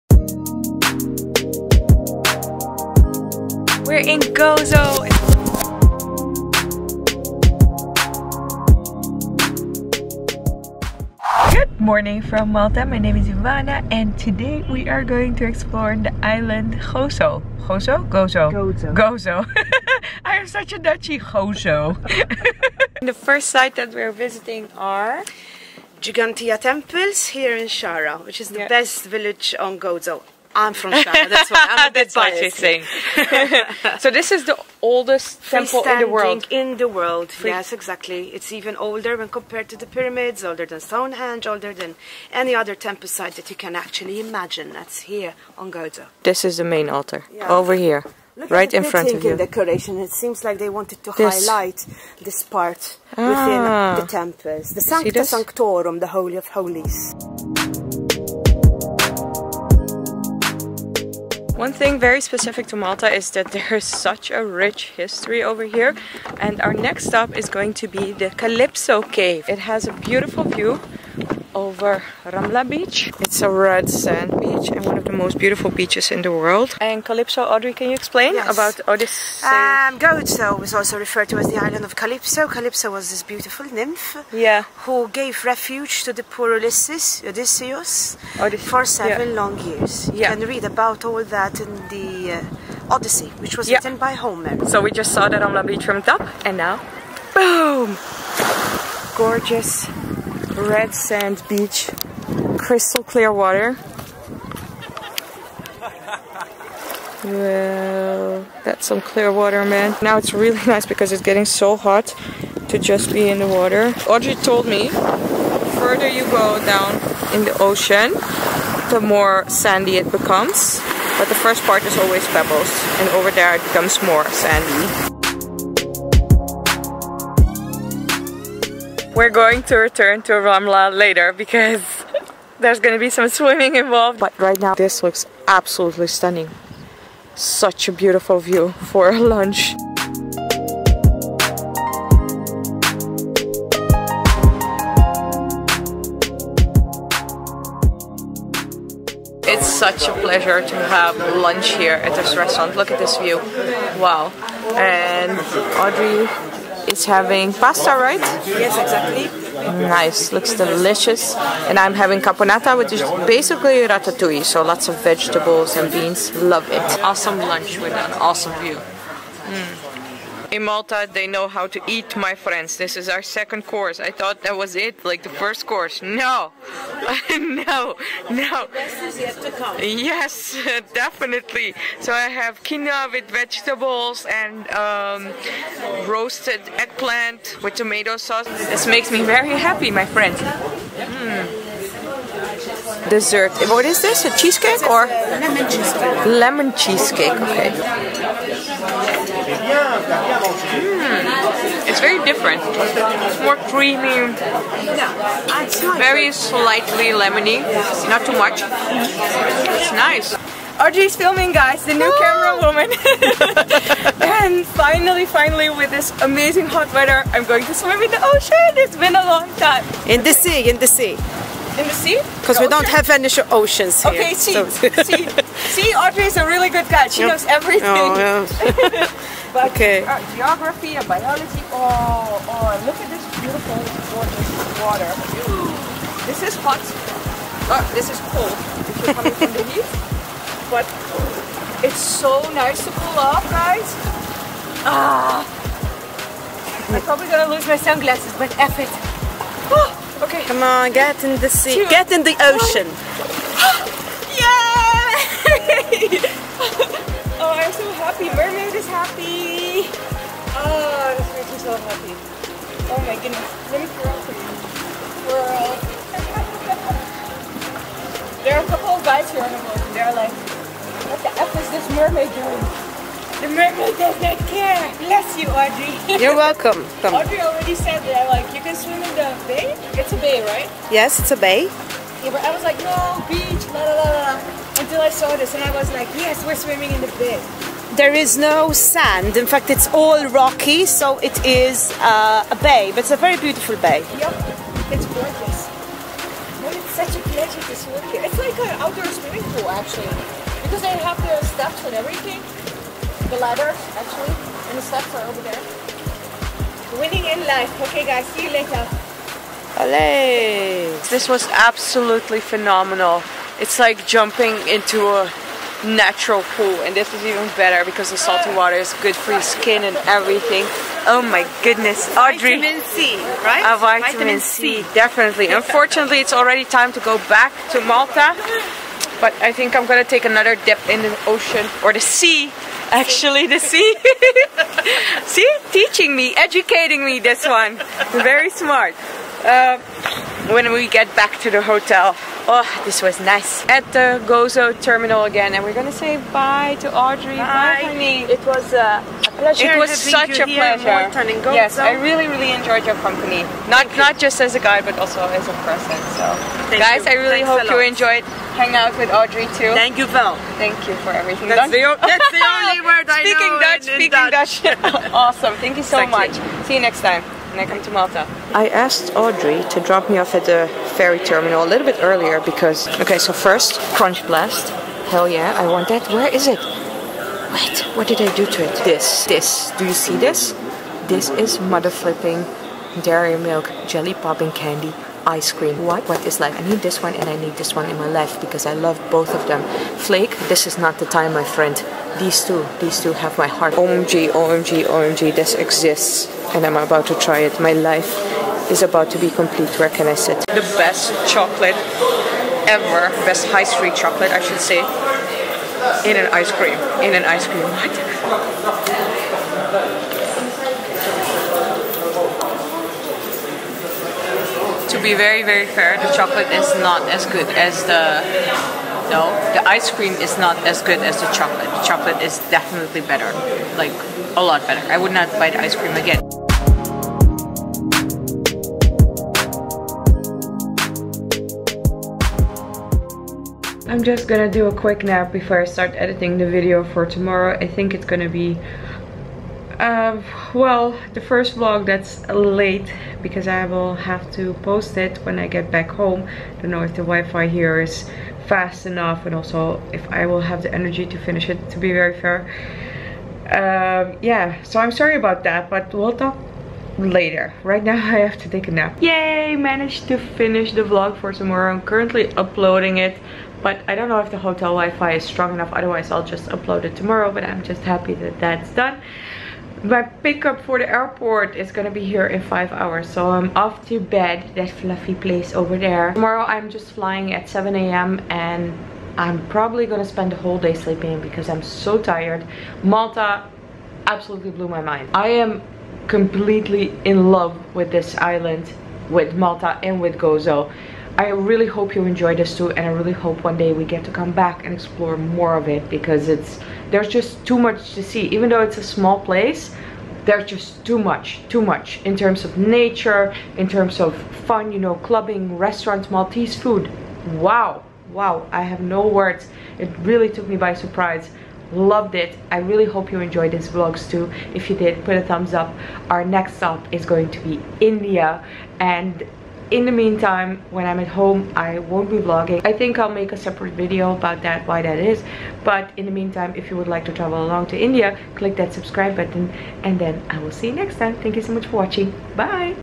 EXPLORING GOZO MALTA DAY 613 | TRAVEL VLOG IV
Exploring Gozo Malta. Our last day on the beautiful island of Malta but we're exploring another island: Gozo! Gozo has so much to offer in terms of history but of course also delicious Maltese food and beautiful beaches. Really fell in love with Malta and Gozo!

If you're on a roll here's the playlist with all the Malta vlogs: